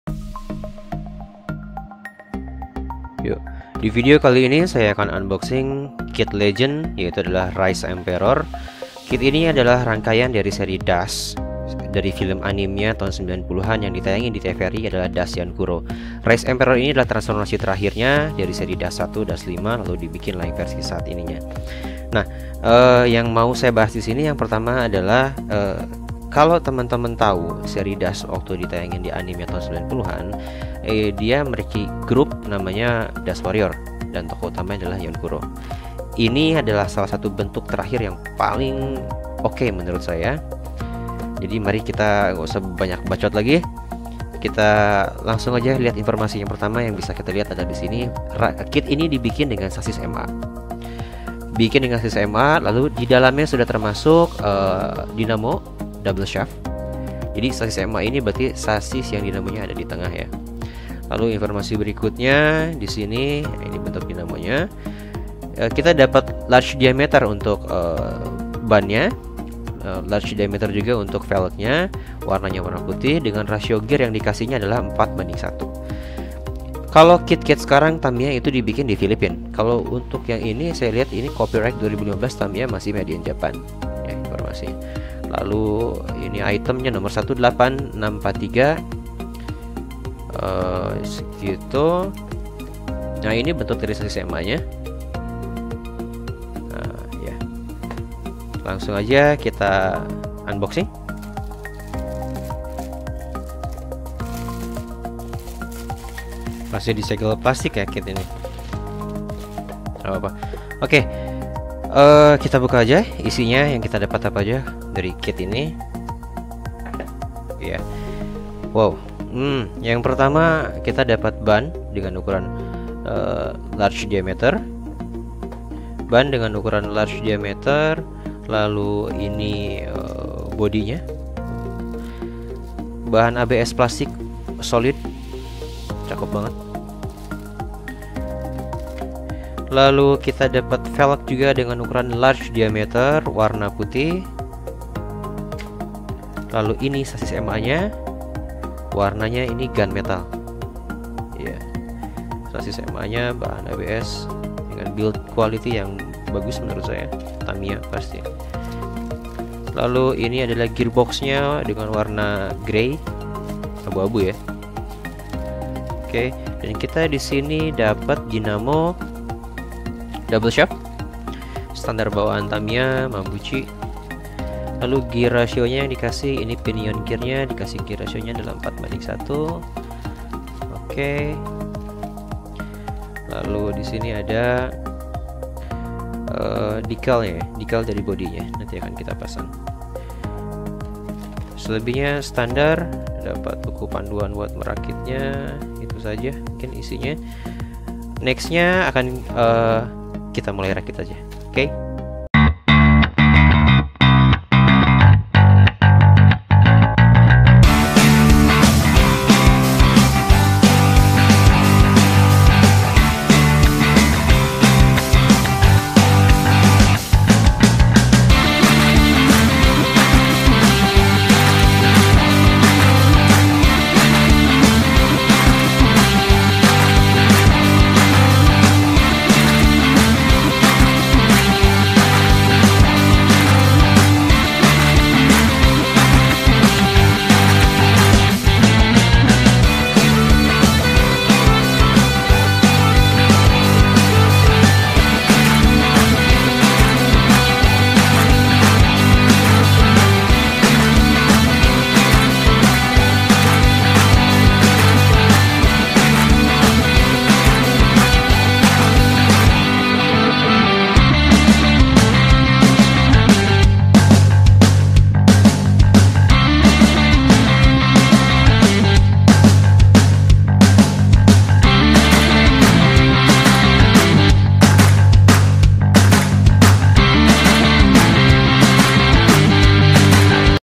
UNBOXING TAMIYA RISE EMPEROR | DASH YONKURO | ミニ四駆 TAMIYA | MINI 4WD
Unboxing Kit Legend
Seri Dash Yonkuro, Rise Emperor
Kit ini udh mulai langka di pasaran..
Tapi masih ada..
Enjoy it.. Go For it..

Best regards..